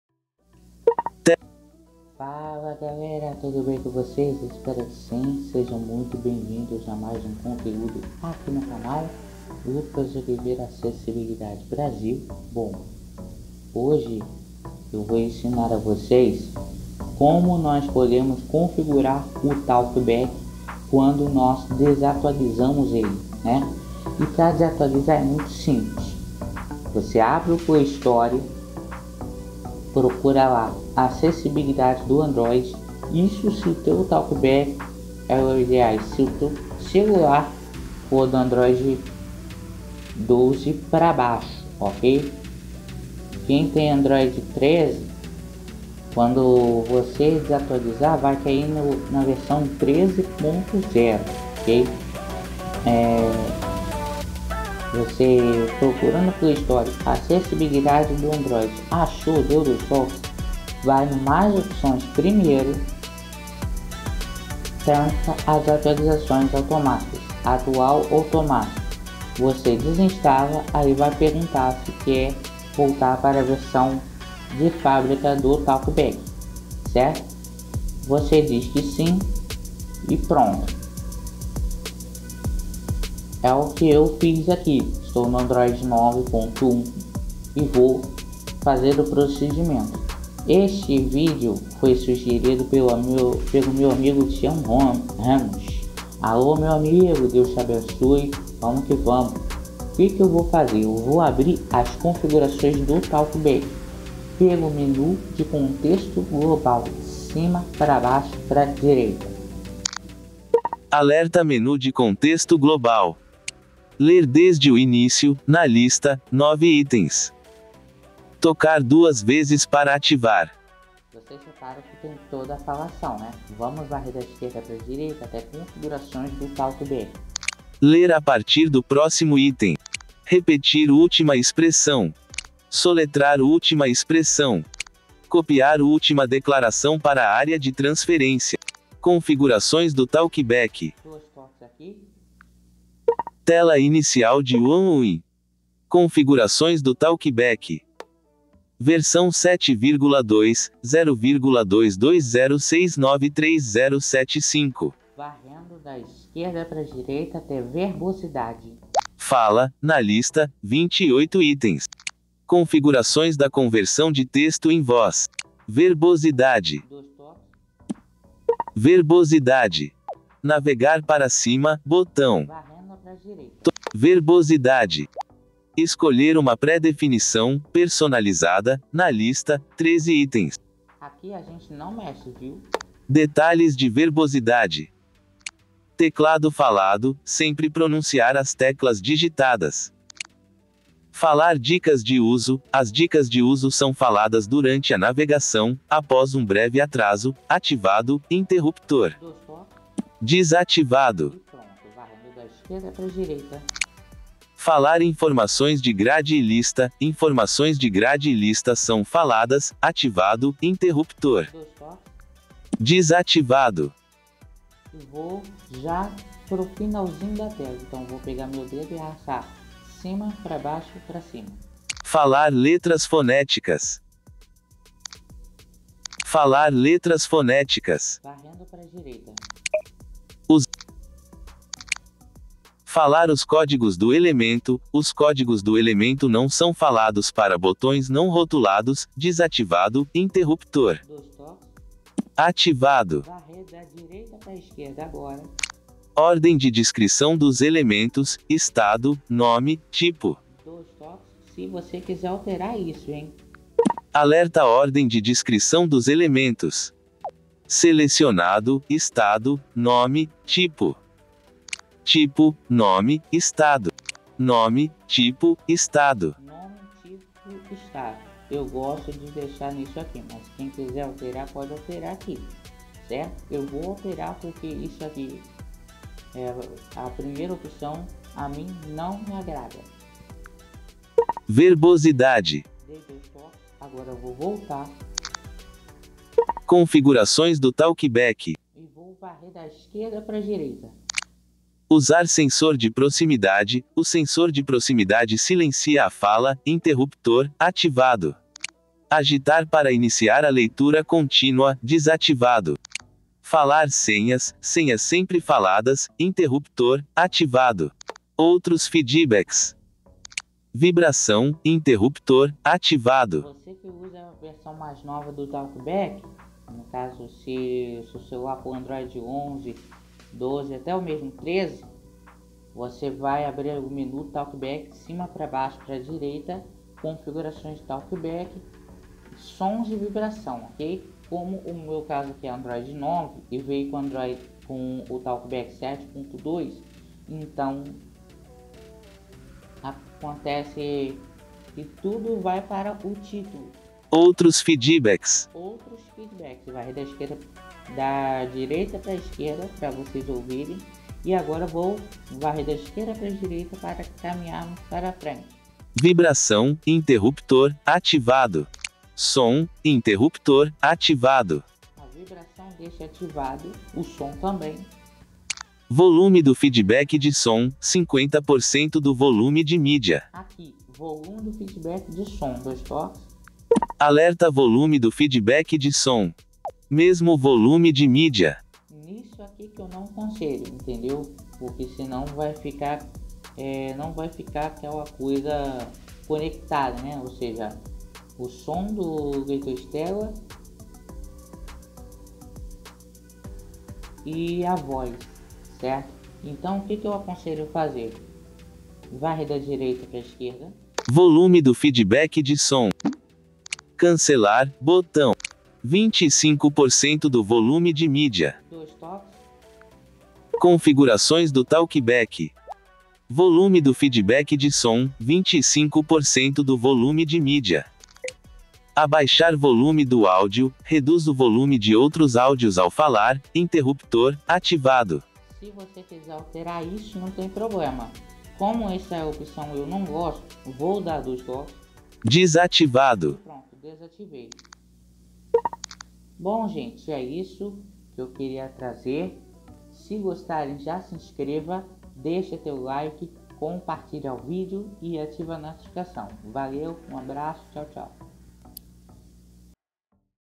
desinstalando atualização do google talkback voltando ao padrão de fábrica
este vídeo foi sugerido pelo meu amigo tião ramos no qual está com problema no google talkback na sua nova versão que é a 140 e vale ressaltar que este método só funcionará com o google talkback ou seja motorola xiaomi xiaomi e lg e demais marcas que utilizam em apenas o google talkback já na samsung do android 11 e superior existe um outro método que é no caso configurações aplicativos talkback mais opções desinstalar atualizações e dá ok sobre o item